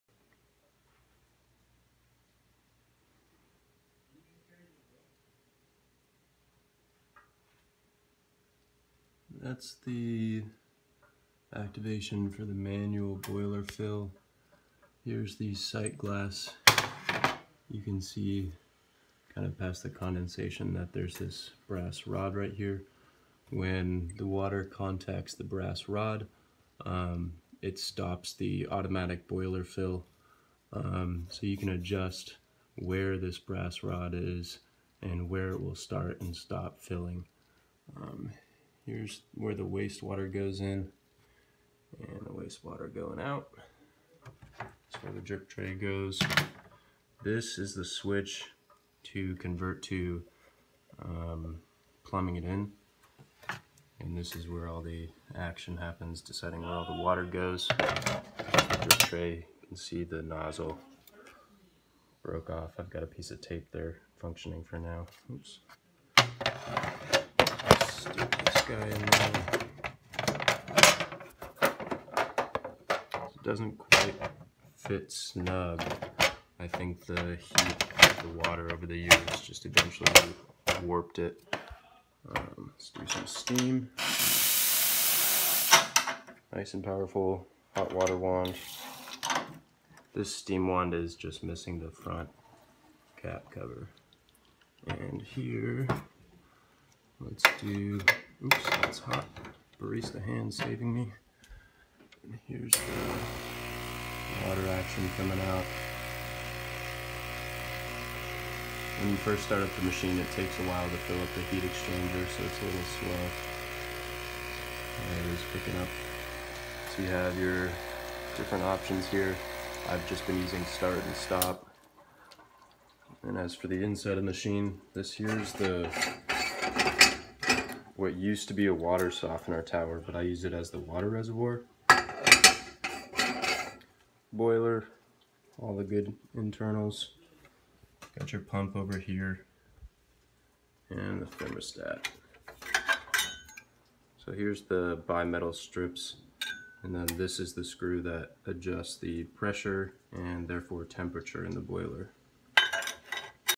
Faema single group
a run through functions and look at the insides. forgot to mention that the main LED on the front does not indicate when the machine is on, it indicates when the boiler is fully heated and at pressure.